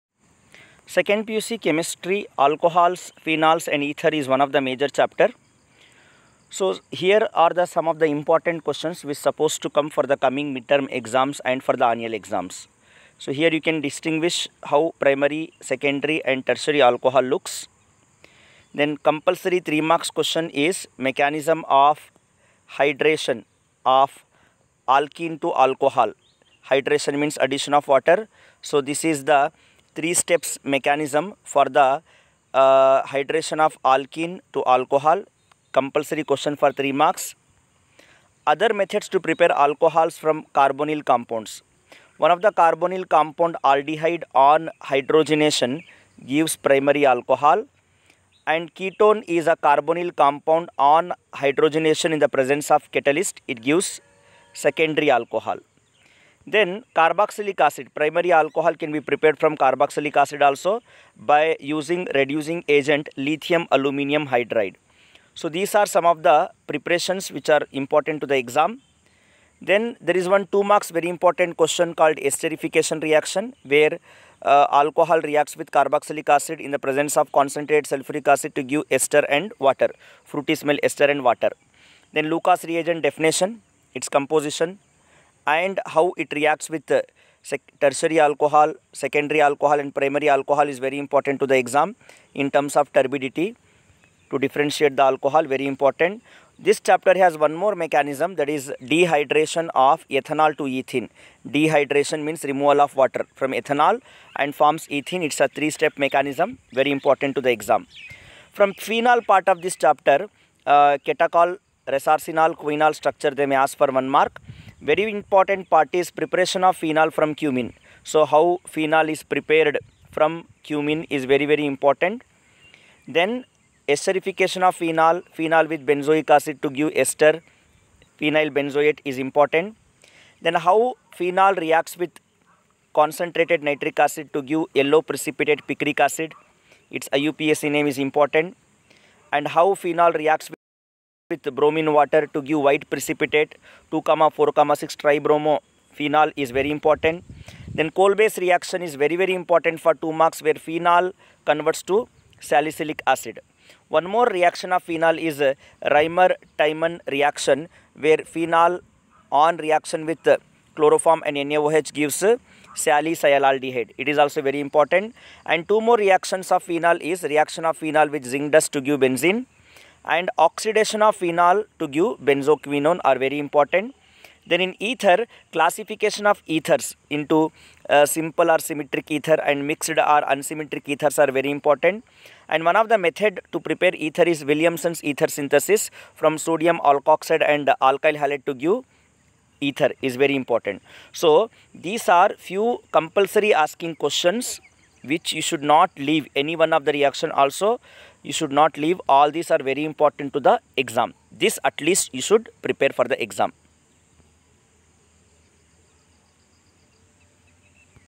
Alcohols phenols and ethers Revision notes | II PUC CHEMISTRY Important topics for exams
Alcohol phenols and ethers very important question answers for the coming exams
2nd PUC chemistry important topics
12th CBSE chemistry organic chemistry reactions